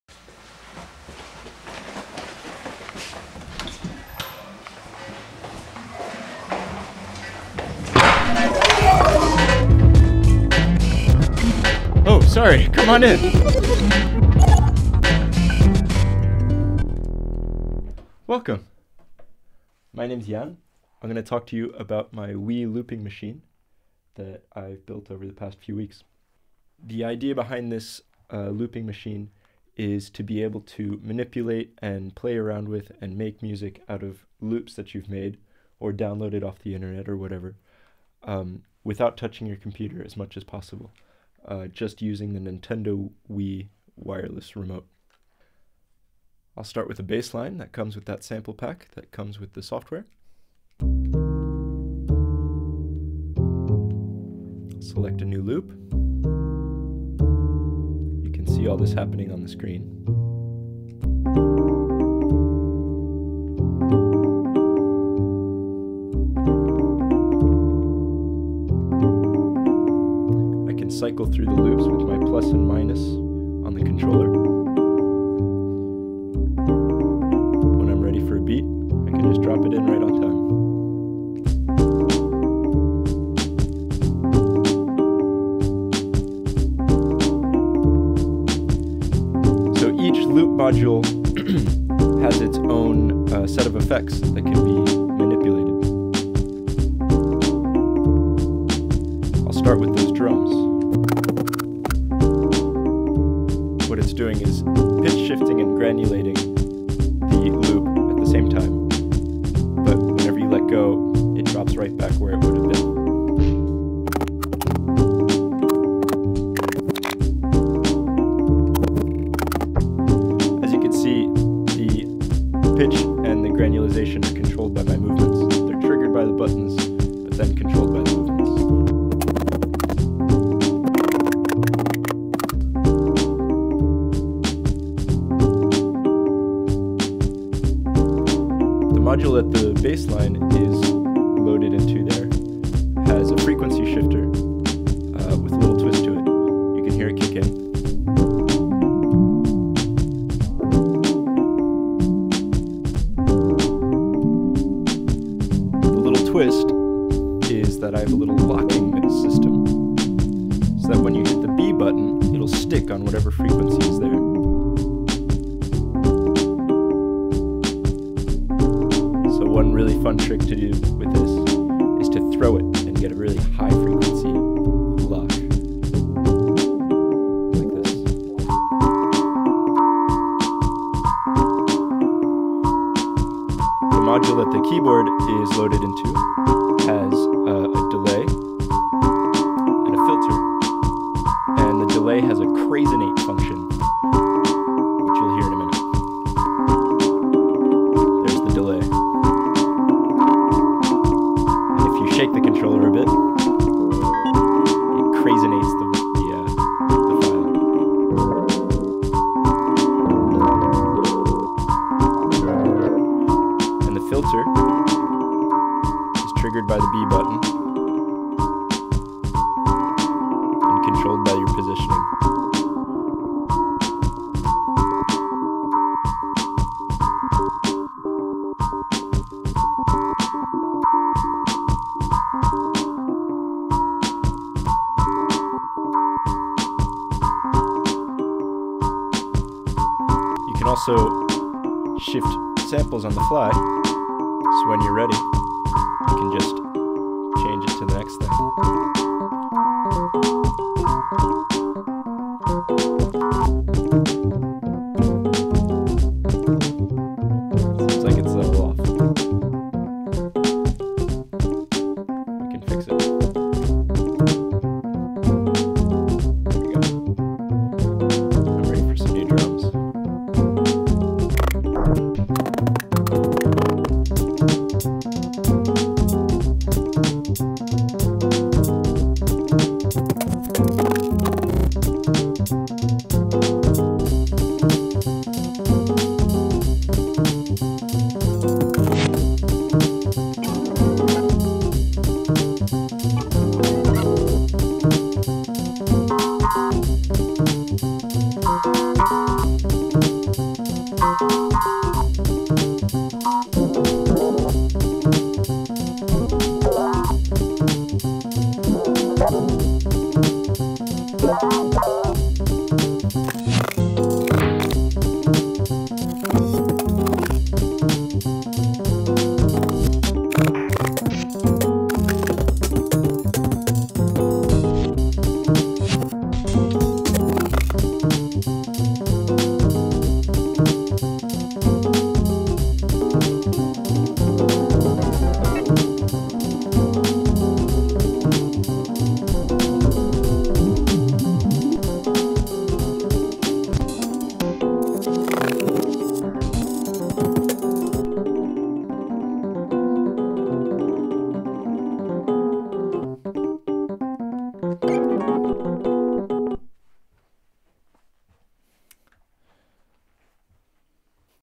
Wii Loop Machine Demo Part 1
A demo of my Wii Loop Machine software, which uses a Nintendo Wii controller to wirelessly controller music on your computer.